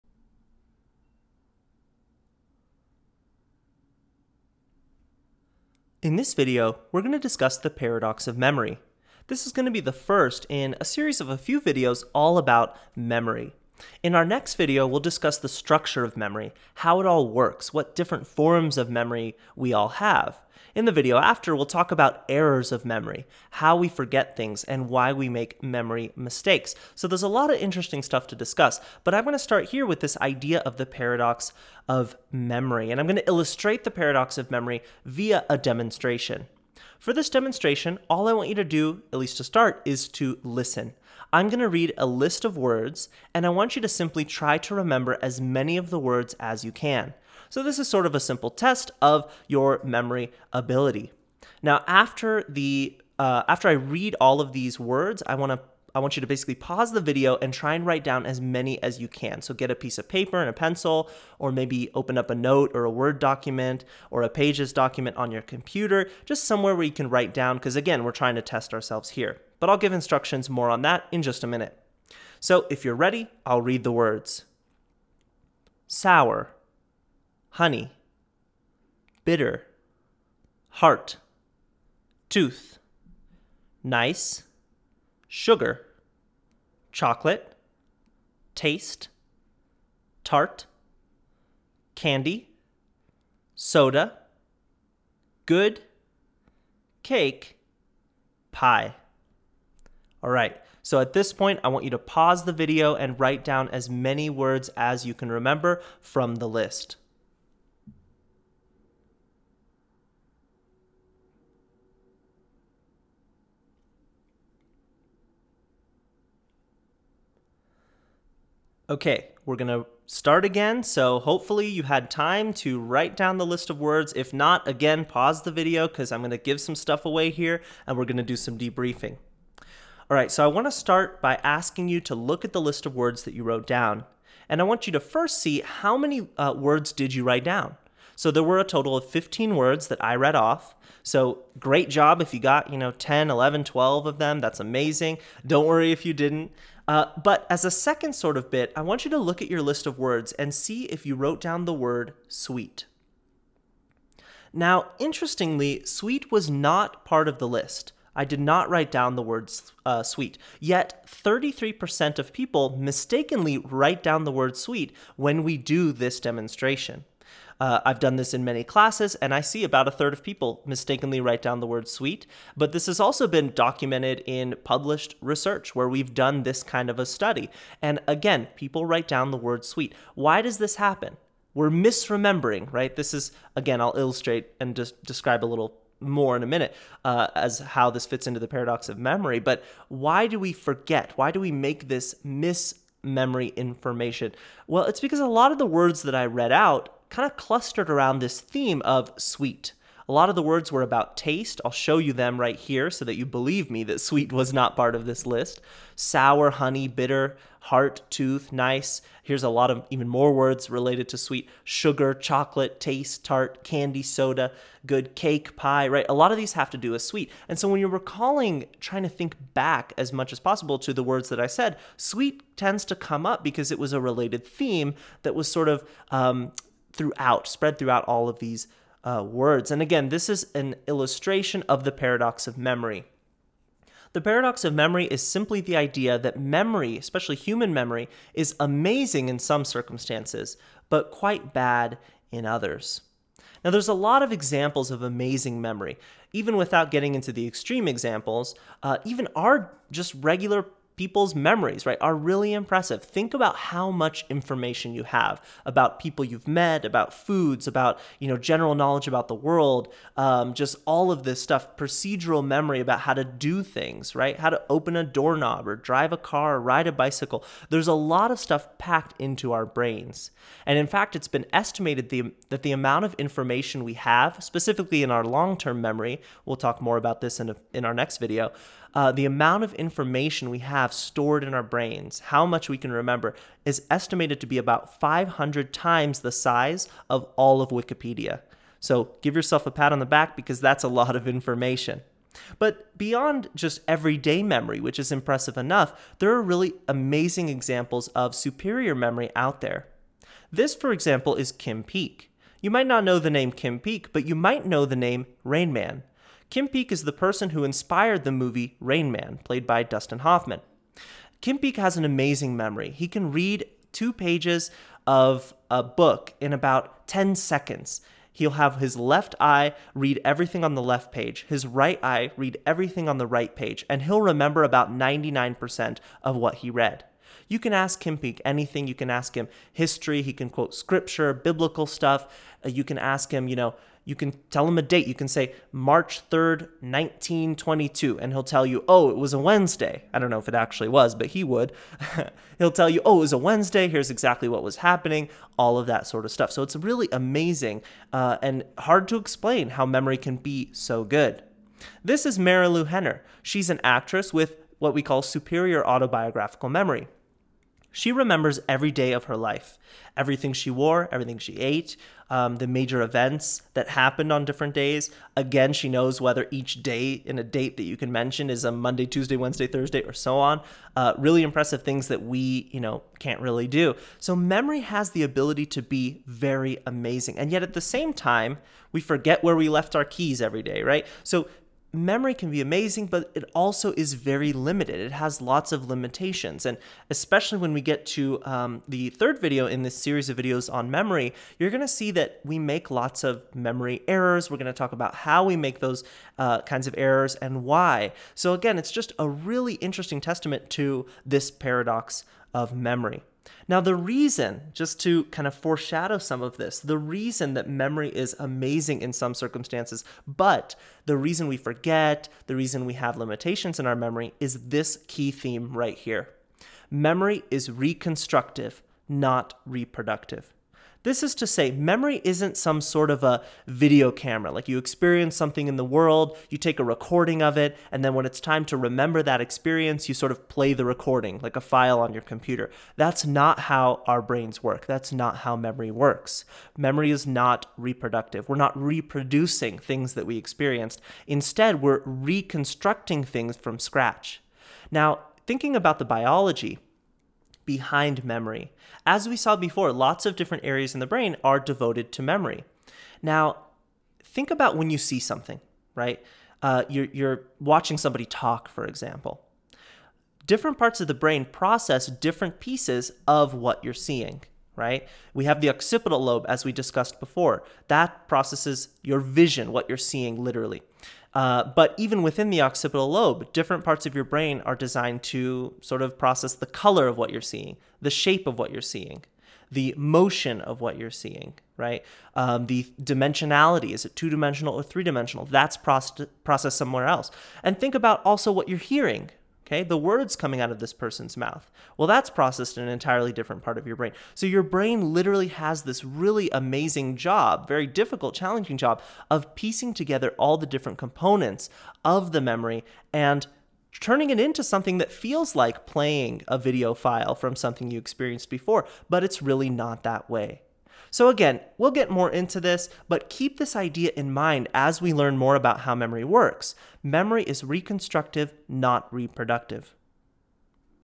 The Paradox of Memory
In this video, we discuss the paradox of memory (see below and in the video for a definition). In our next two videos, we'll learn about how memory works and why we forget things.

The Paradox of Memory: Memory is amazing in some circumstances, but quite bad in others; this is because memory is reconstructive, not reproductive.